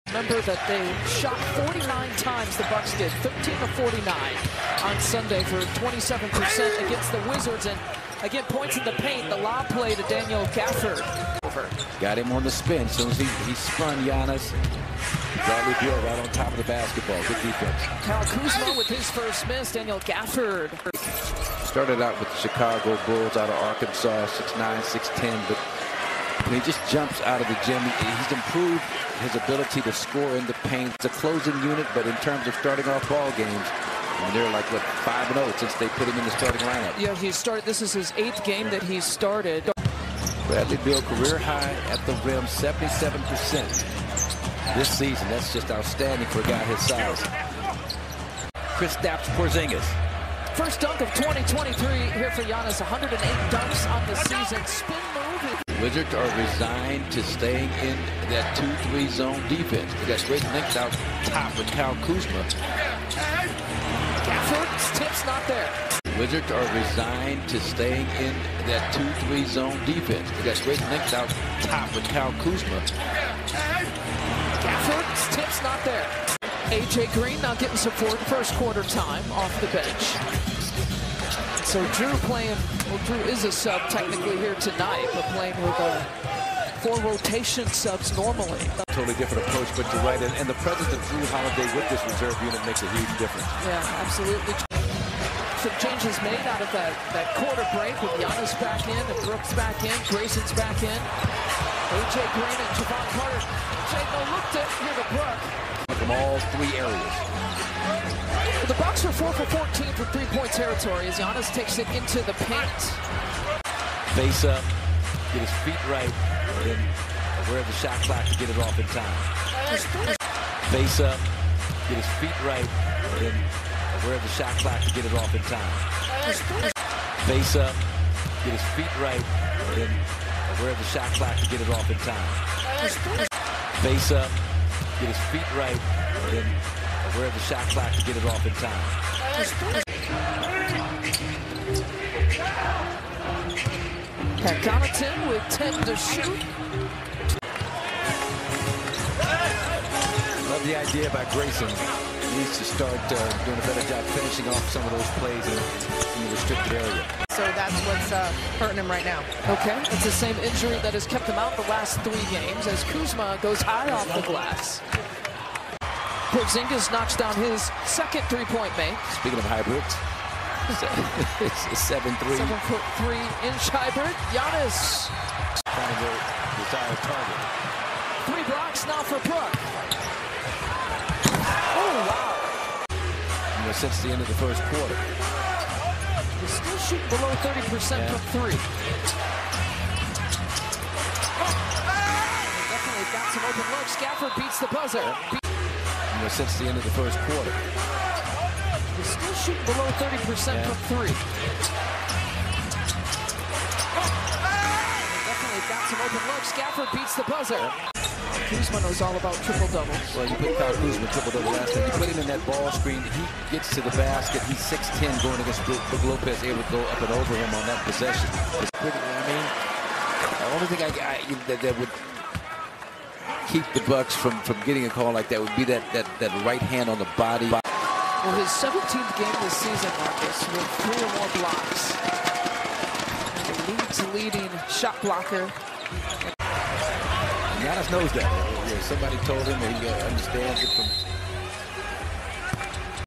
Daniel Gafford 11 PTS 12 REB: All Possessions (2023-01-03)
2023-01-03 Milwaukee Bucks vs Washington Wizards: Player Highlights Daniel Gafford
    11 PTS, 12 REB, 2 AST, 0 STL.
        0 of 0 from 3.
        4 of 7 FGs in PT29M03.00S minutes.